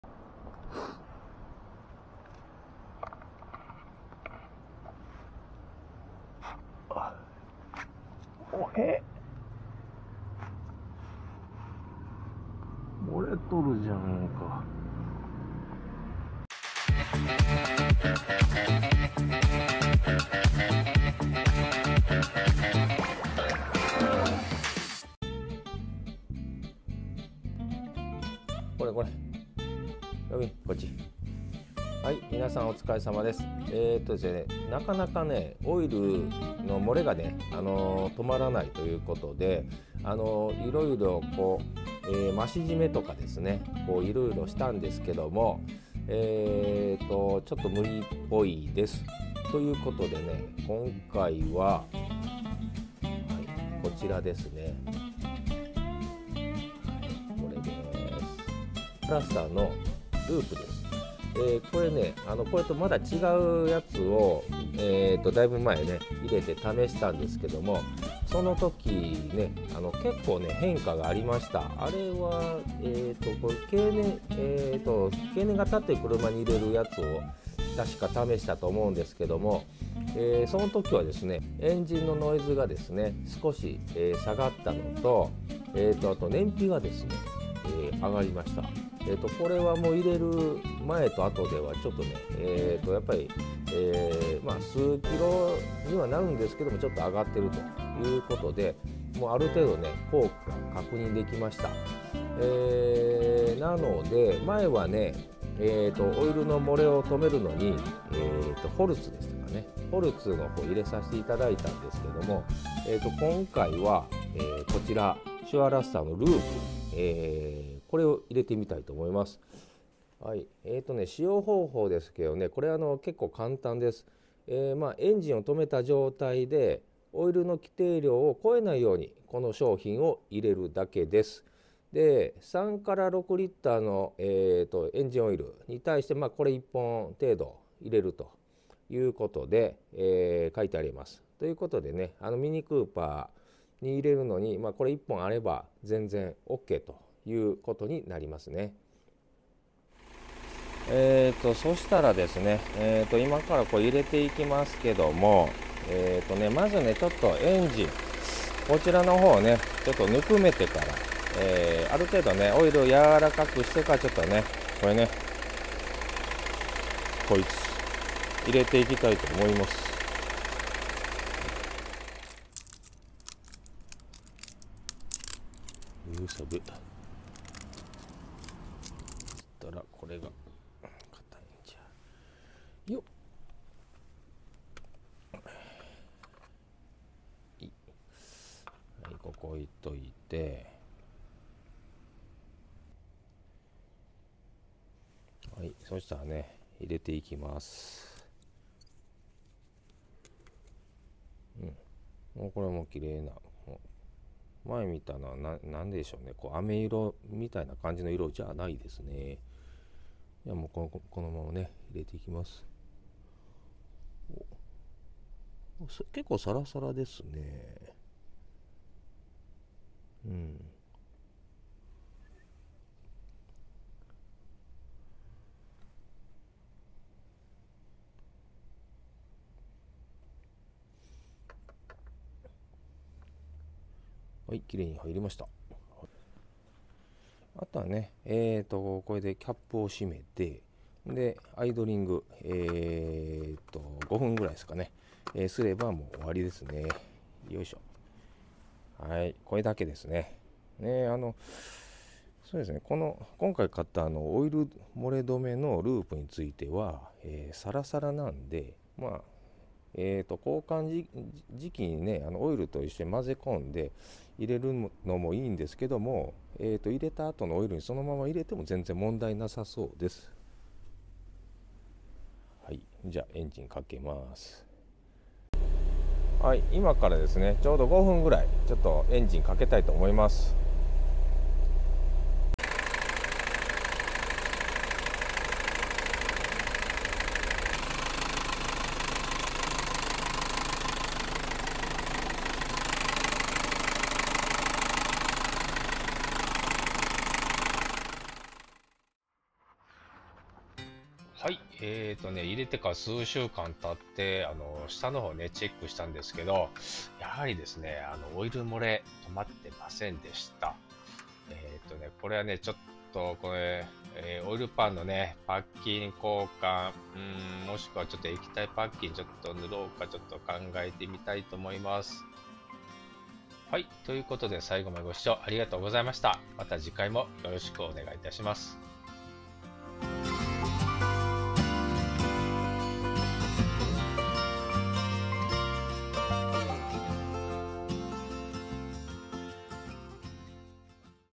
[DIYレストア]R56MINIクーパーSにシュアラスターｌｏｏｐのストップリークを入れてみた！
中々止まらないオイル漏れ！！！

色々やってるけど効果ない！
でもパッキン交換は大変です。
でっ！

またまた、オイル漏れ止め剤入れてみたよ！
オイル漏れ止まれよ！

今回購入したシュアラスター エンジンオイル添加剤 LOOP エンジンストップリーク SurLuster LP-45 オイル漏れ改善&防止